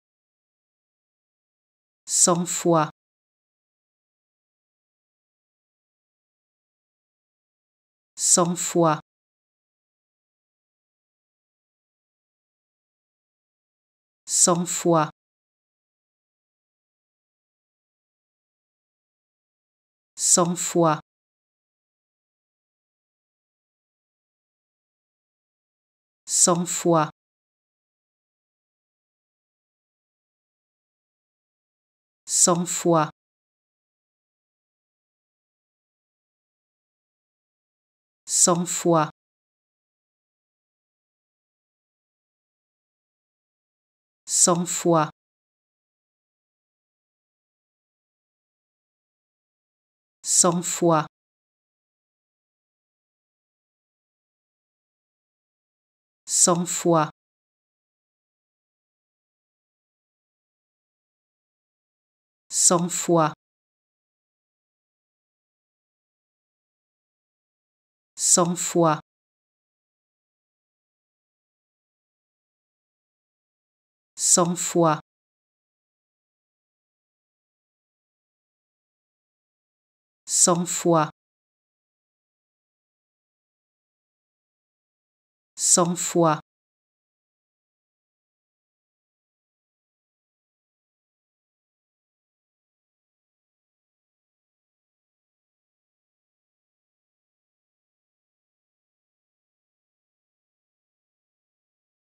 How to Pronounce ''Cent fois'' (A hundred times) Correctly in French
Learn how to say and properly pronounce ''Cent fois'' in French with this free pronunciation tutorial.